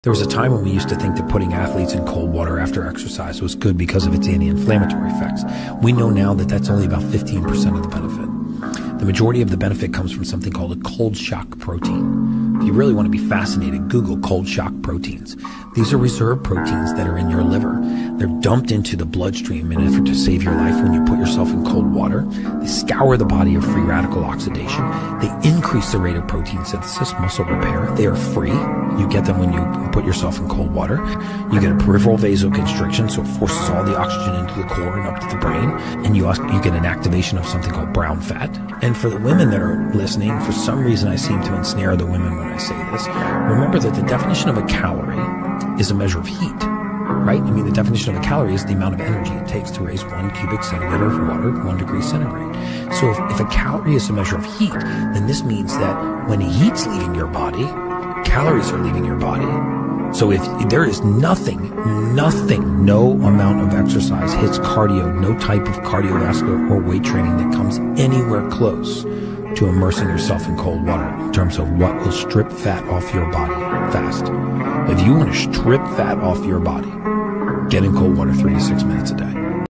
Cold shock protein - Motivational Speech
This motivational video emphasizes the importance of setting and staying focused on goals to achieve success. With keywords such as "motivation," "self-improvement," and "goal setting," the video encourages viewers to prioritize their aspirations and work hard to overcome challenges. The video also promotes the idea of resilience, dedication, and perseverance as critical components of achieving goals. Motivation Inspiration.